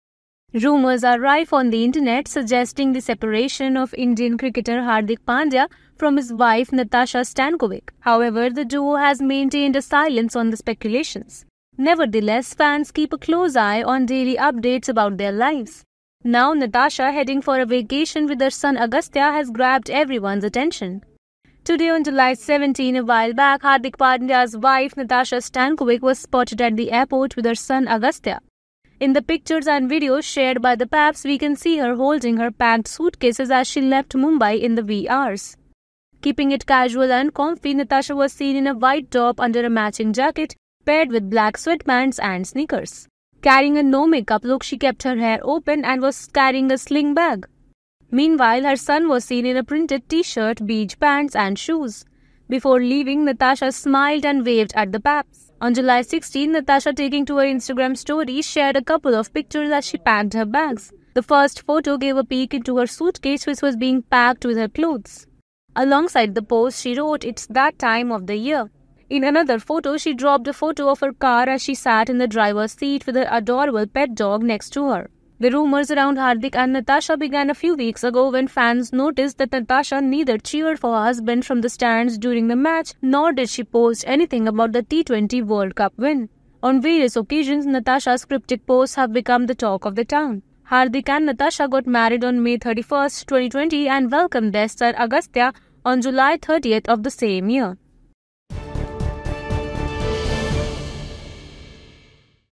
Natasa Stankovic was spotted at the airport with her son Agastya, leaving for a vacation!
Natasa Stankovic was spotted at the airport with her son Agastya, leaving for a vacation amid rumors of a split with Indian cricketer Hardik Pandya. Fans continue to follow their updates closely.